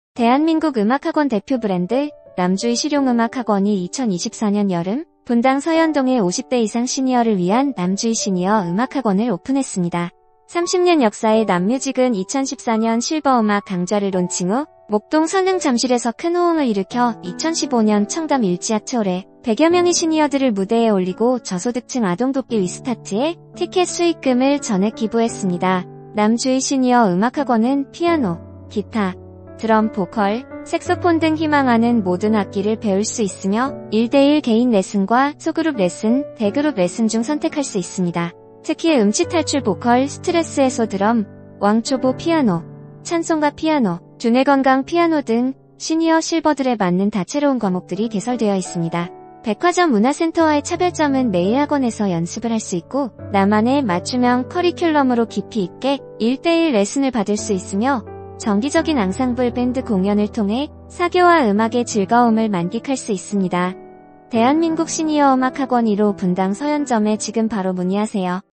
2024년 7월, 시니어를 위한 새로운 무대가 열립니다! [전화문의 ]
대한민국 음악학원 대표 브랜드 '남주희실용음악학원'이
2024년 여름 분당 서현동에 50대 이상 시니어 전용 '남주희시니어음악학원'을 오픈했습니다.

30년 역사를 갖는 남뮤직은 2014년 '실버음악 강좌'를 론칭 후 목동 선릉 잠실에서  큰 호응을 일으켜 2015년 청담 일지아트홀에 100여명의 시니어들을 무대에 올리고 '저소득층 아동돕기 위스타트'에 티켓 수익금을 전액 기부 했습니다.

남주희시니어음악학원은 피아노.기타.드럼.보컬.색소폰 등 희망하는 모든 악기를 배울 수 있으며 1:1 개인 레슨과 소그룹 레슨(3~4명) 대그룹 레슨(5~6명)중 선택할 수 있습니다.

특히 음치탈출 보컬, 스트레스 해소 드럼, 왕초보 피아노, 찬송가 피아노,두뇌건강 피아노 등 시니어,실버들에 맞는 다채로운 과목들이 개설되어 있습니다.

백화점 문화센터와의 차별점은 매일 학원에서 연습을 할 수 있으며 나만의 맞춤형 커리큘럼으로 깊이 있게 1:1 레슨 받을 수 있으며 정기적인 앙상블 밴드 공연을 통해 사교와 음악의 즐거움을 만끽할 수 있습니다. 대한민국 시니어음악학원 1호 분당 서현점에 지금 바로 문의하세요.

 ▶ 자세한 문의사항은 카카오톡 상담(ID : nammusic)
or 전화상담(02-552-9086) 부탁드립니다!